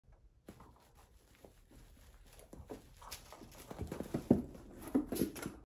Video of adoptable pet named Soup
Soup is an adoptable pet with The Dancing Cat in San Jose CA. Please visit their